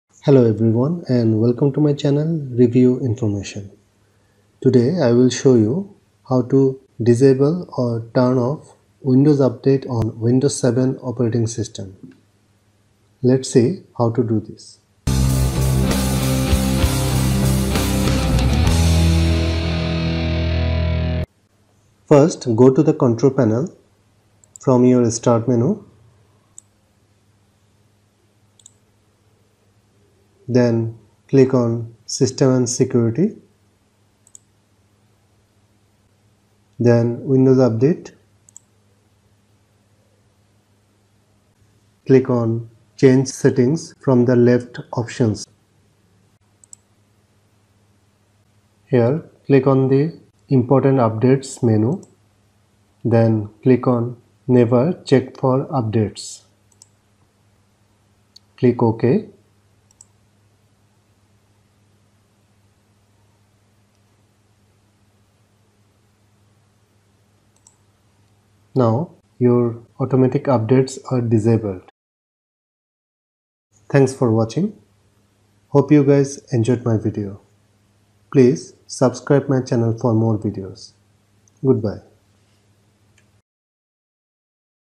How To Turn Off / Disable Automatic Updates In Windows 7
In This Video I Will Show You, How To Turn Off / Disable Automatic Updates In Windows 7.